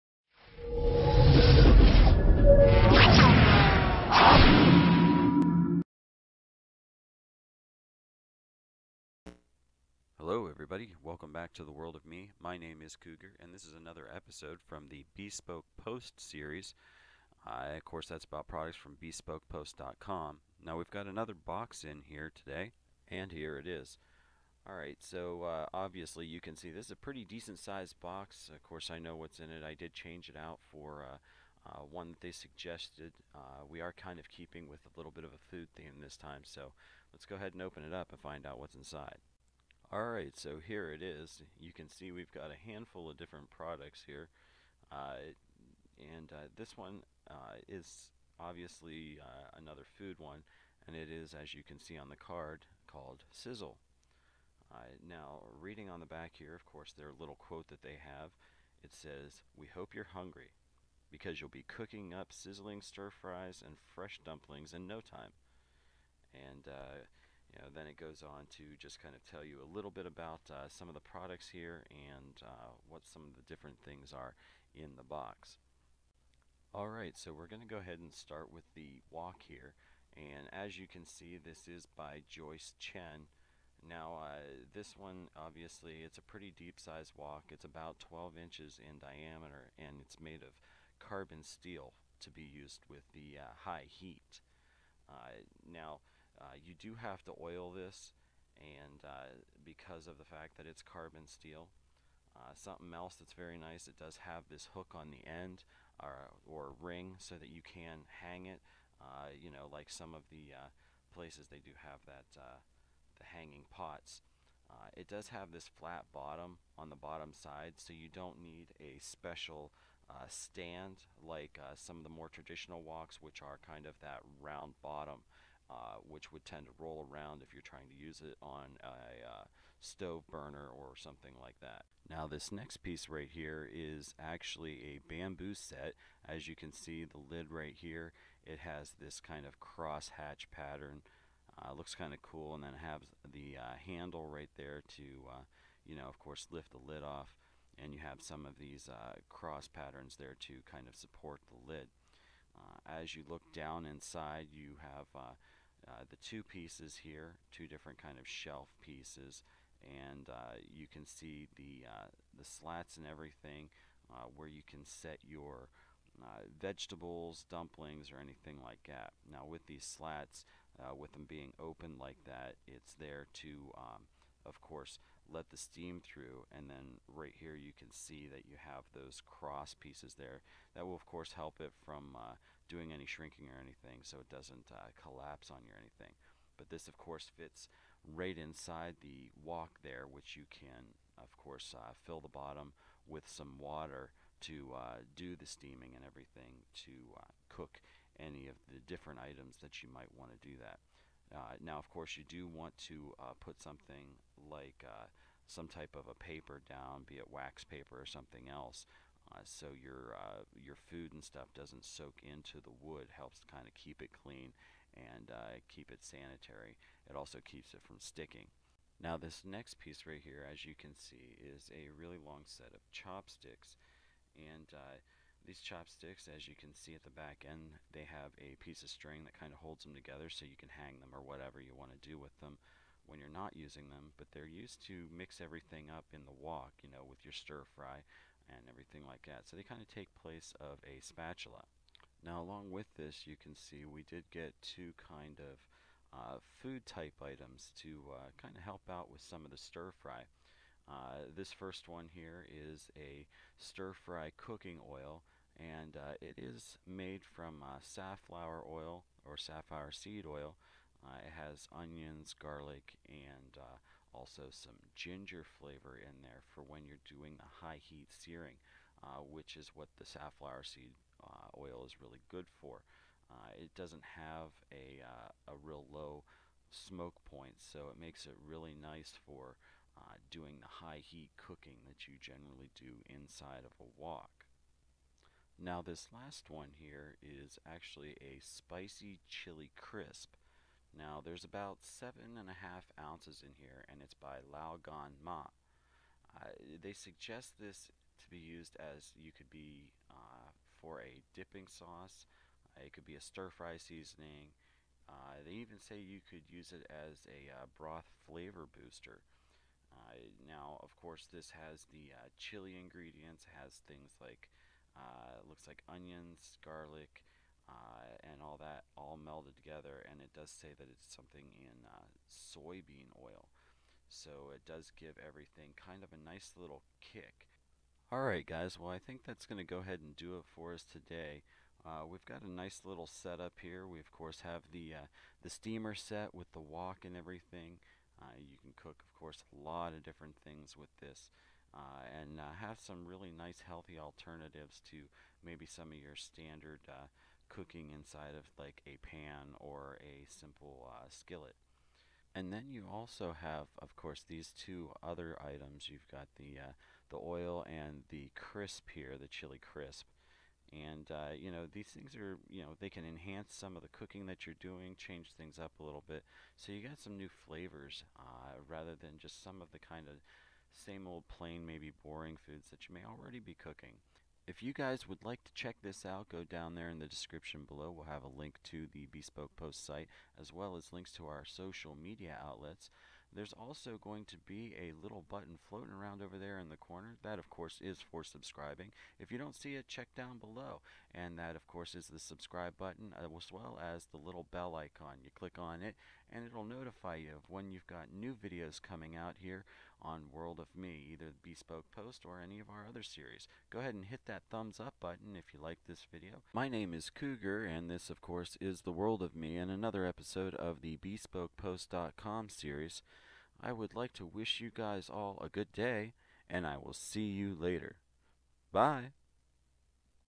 Sizzle - BeSpoke Post
Asian cooking made easier or cook your regular dishes with this wok and bamboo steamer set. High quality carbon steel wok, two bamboo trays with lid, chili mix, stir fry oil, and a set of extra long chop sticks.